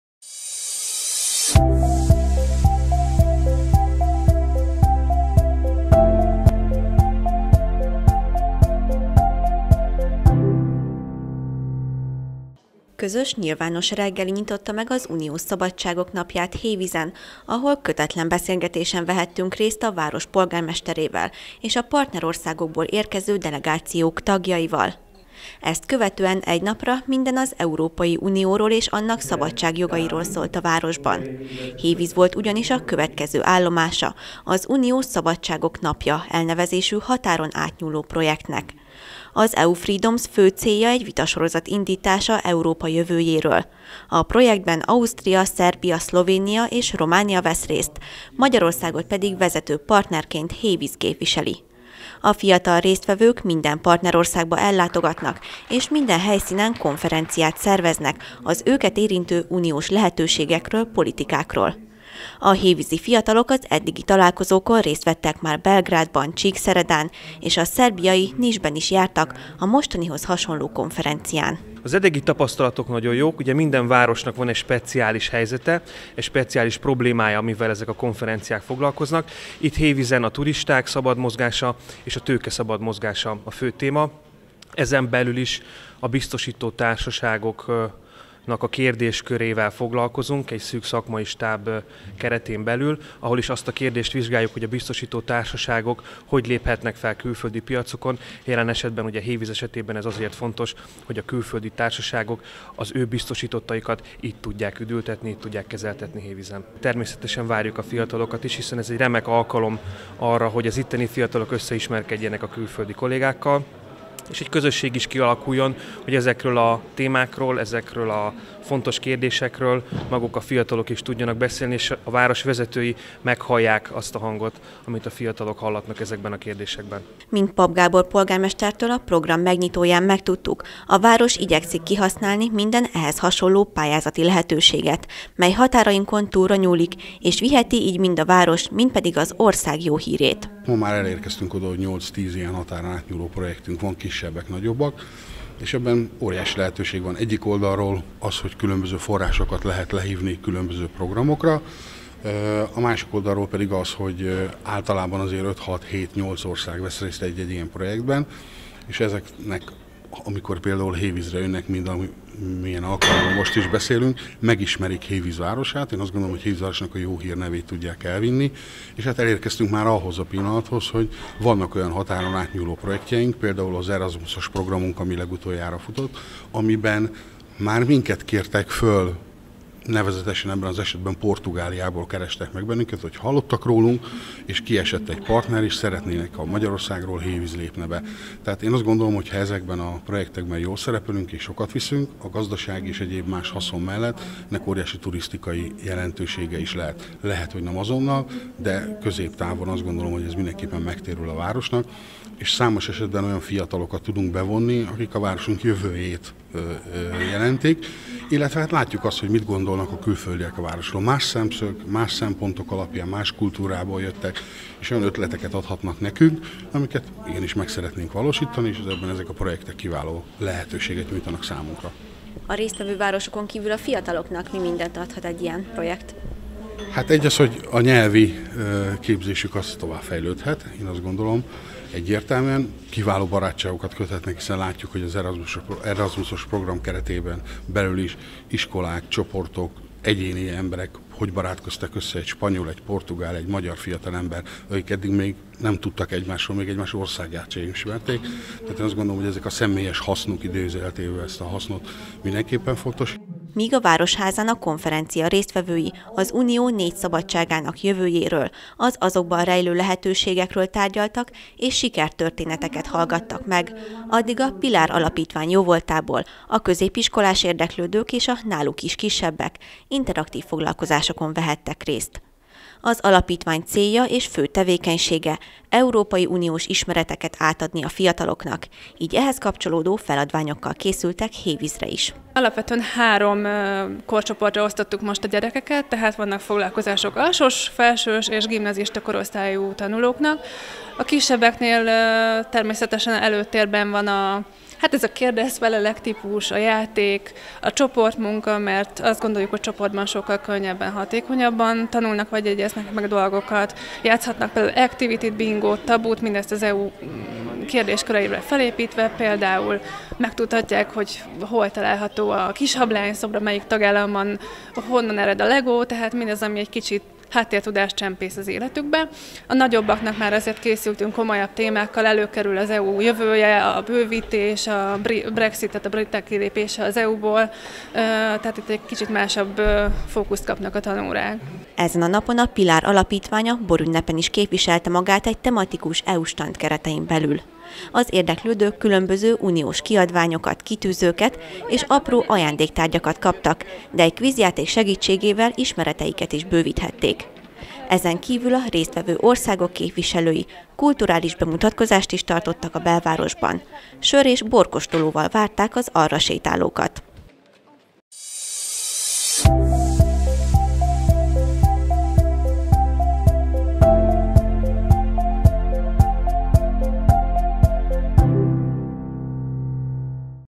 Egy nap az Európai Unió jegyében
Hévíz volt a soron következő állomása az „Uniós Szabadságok Napja” határon átnyúló projektnek.